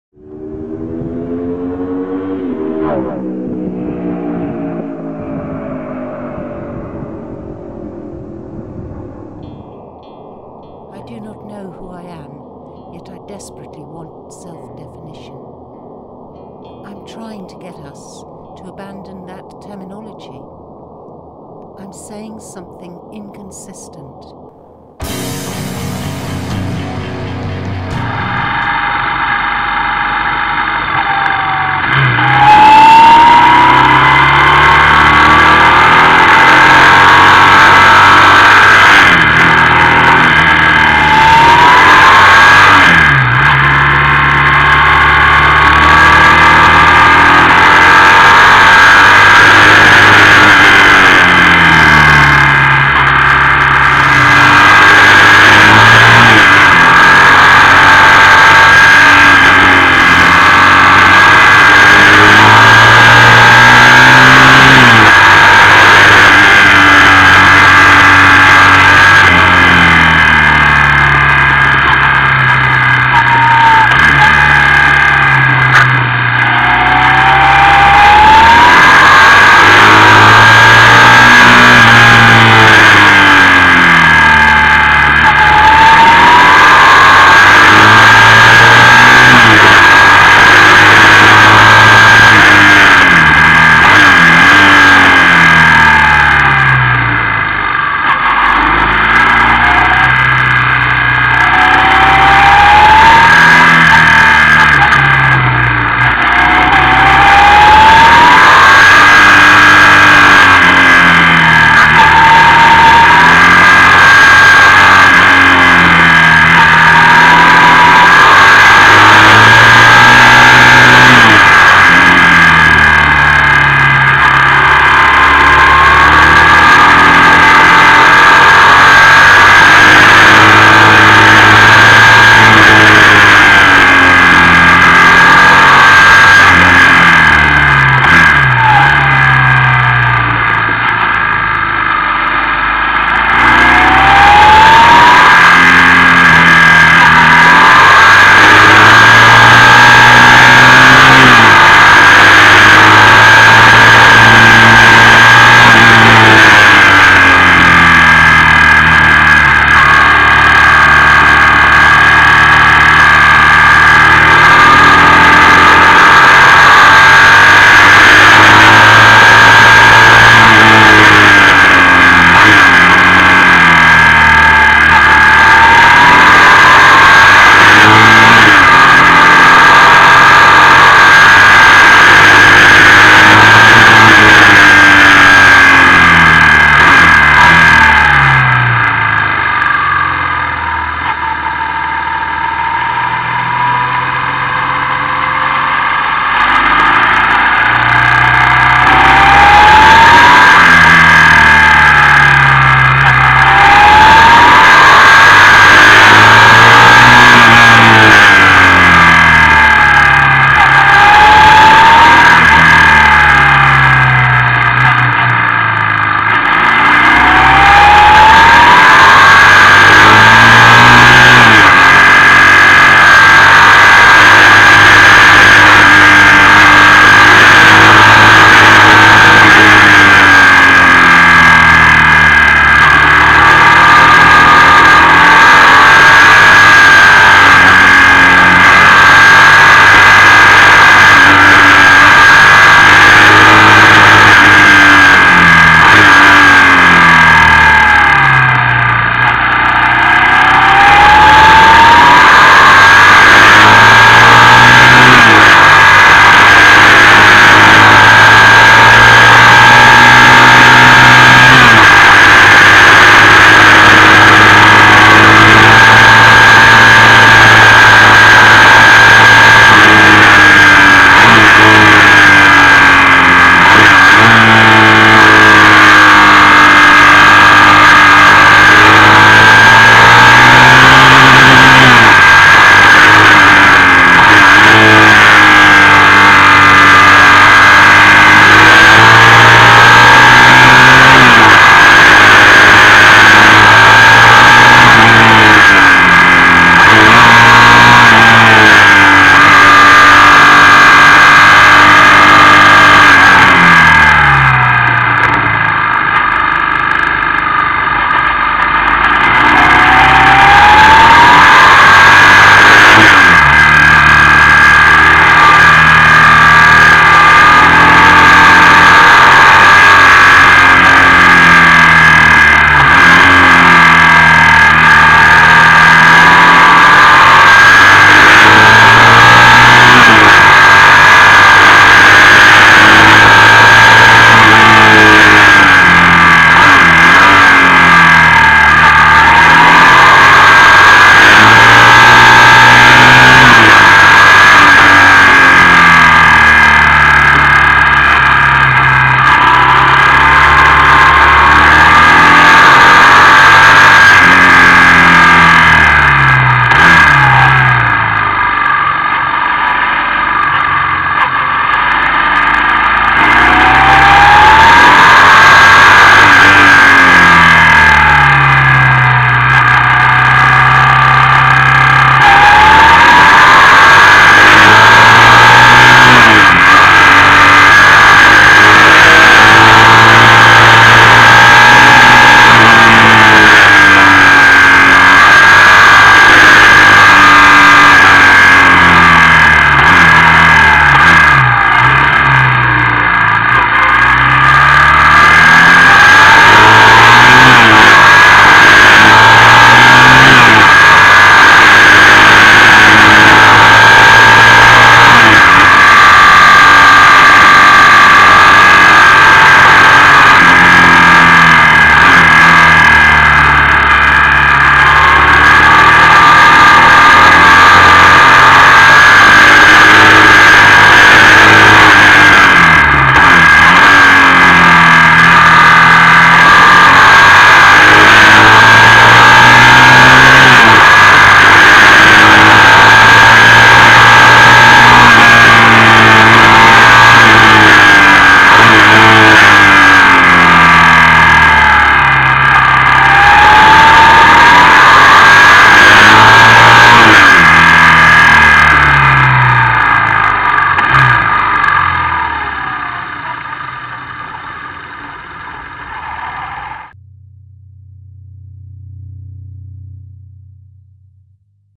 El Topcase gemütlich am Schauinsland
El Topcase gemütlich am Schauinsland - oder wie man eine BMW R1100RS korrekt bewegt...
Bitte HD Mode einschalten